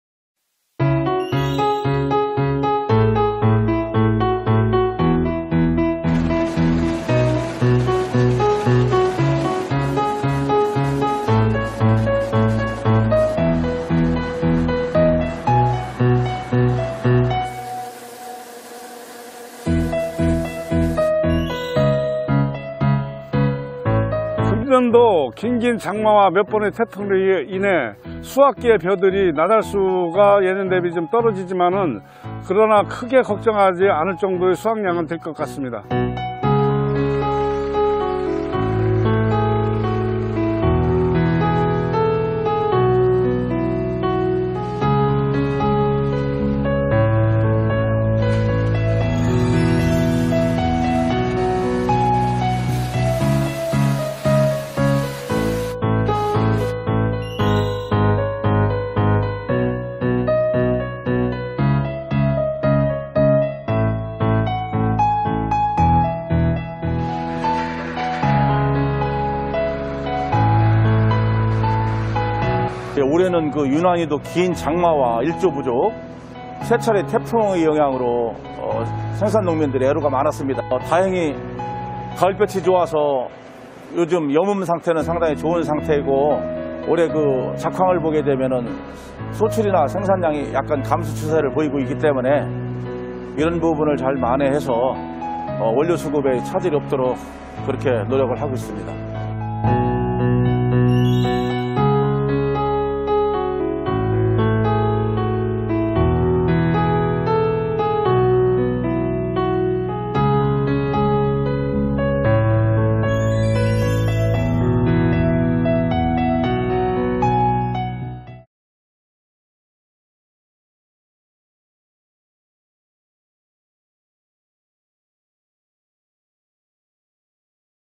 장마와 태풍 피해 걱정하지 마세요! '쌀' 올해도 이상 '무' | 쌀 수급 안정 대책 | 농식품부 News
코로나, 태풍 등 올 한해 쌀농사를
걱정하신 분들이 많으실텐데요.

다만, 크게 걱정할 사항은 아니라고 합니다.

정부의 양곡 재고량은 95만톤에
올해 35만톤을 매입하여
총 130만톤으로
쌀 공급여력은 충분한 상황입니다.

Hello, this is The Official Youtube Channel of MAFRA(Ministry of Agriculture, Food and Rural Affairs), Republic of Korea